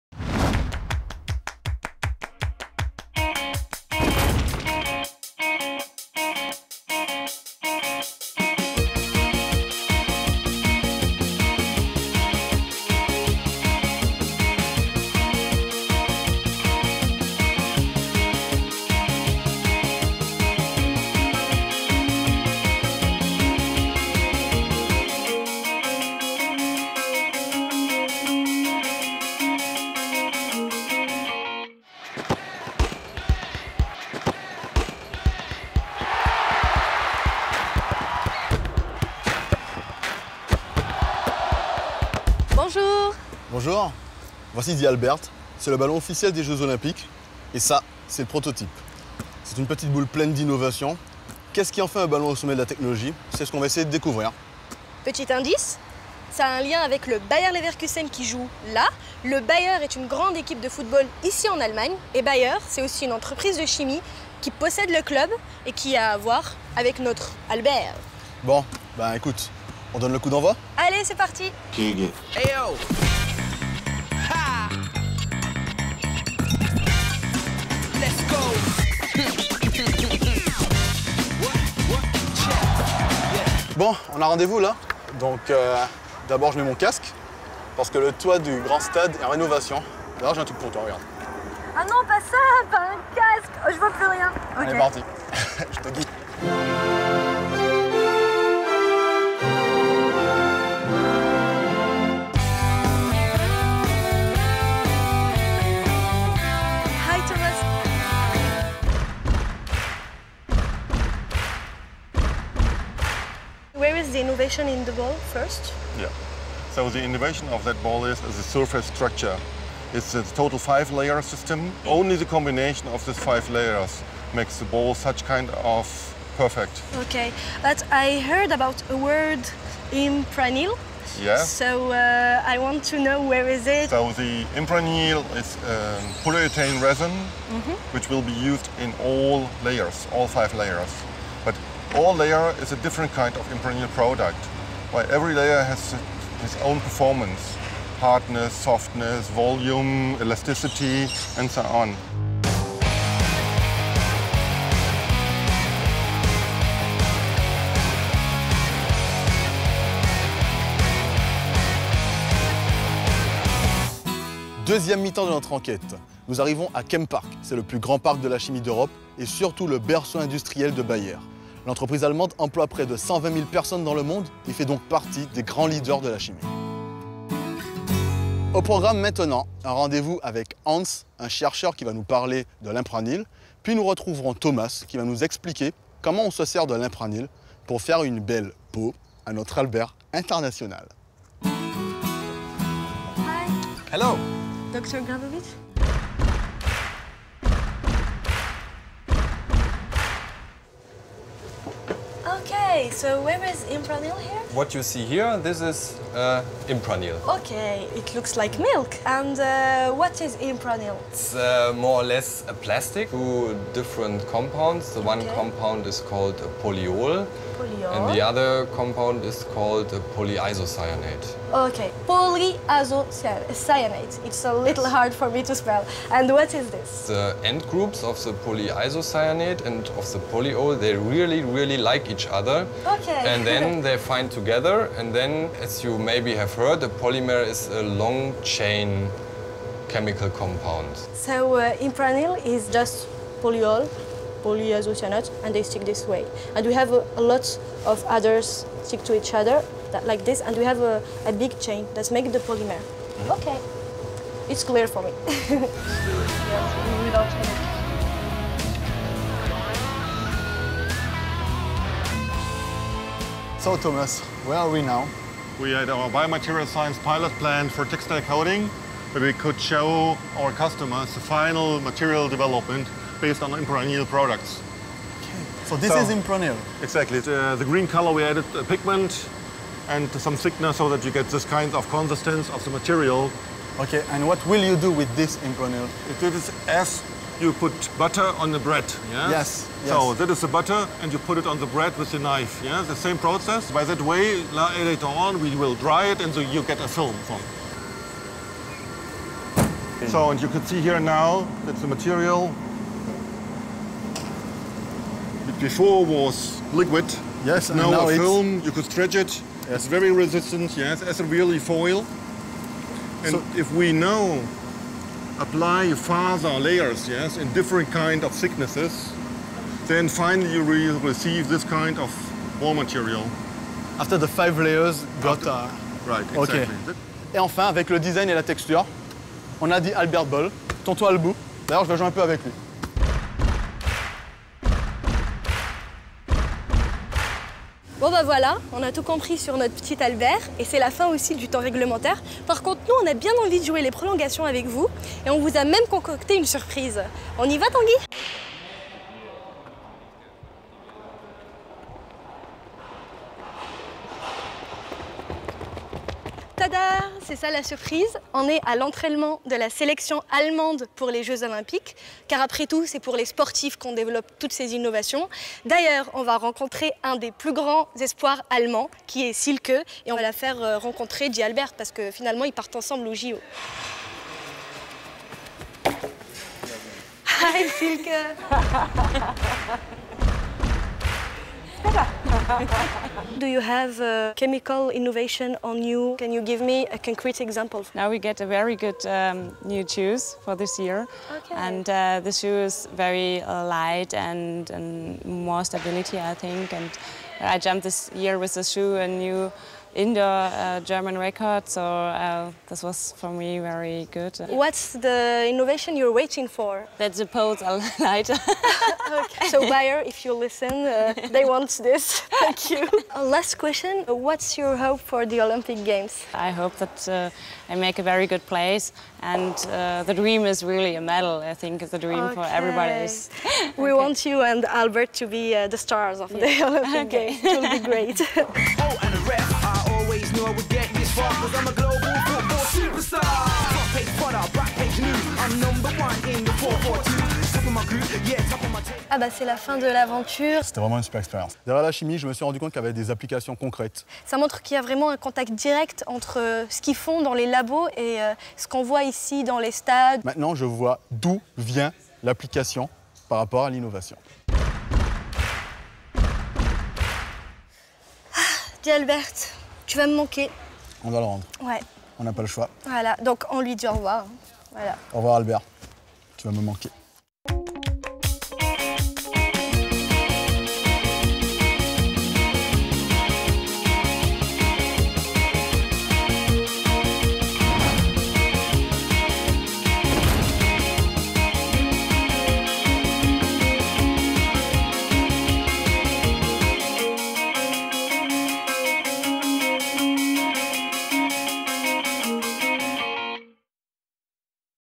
LA CHIMIE ET LES BALLONS DE FOOT - ChemicalWorldTour 2
Chemical World Tour Saison 2 - Episode 3 : Sara et Tanguy partent à la découverte du ballon de football le plus innovant de sa génération.
Direction Leverkusen en Allemagne chez BAYER !